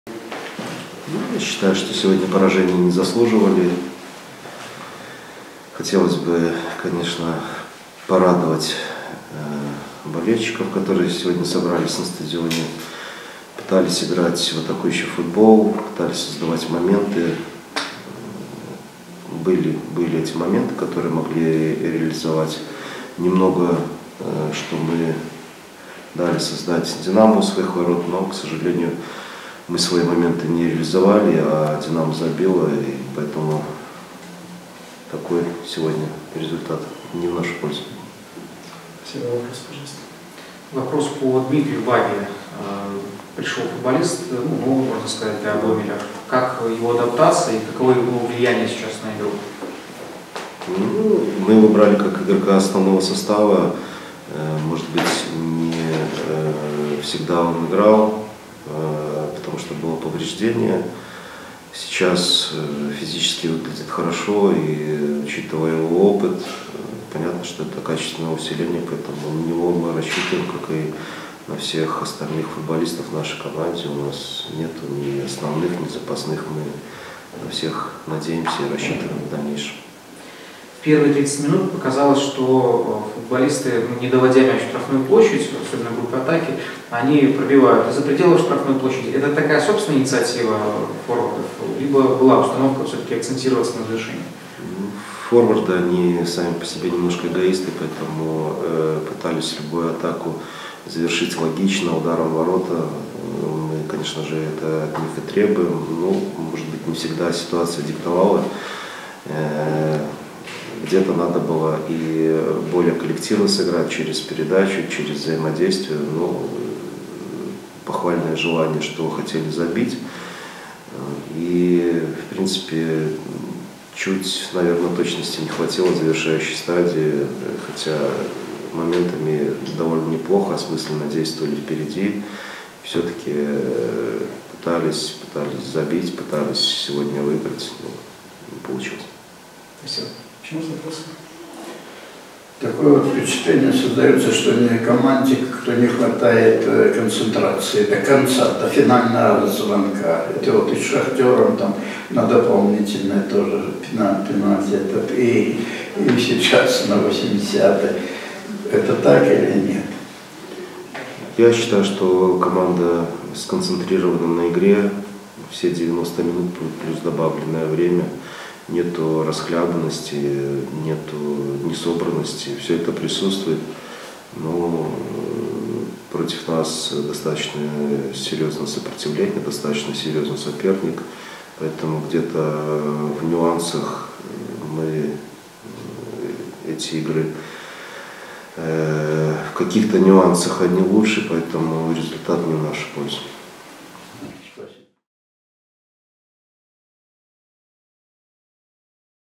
Владимир НЕВИНСКИЙ | Комментарий после матча с минским «Динамо»
"Зелено-белые" со счетом 0:1 проиграли в 7 туре чемпионата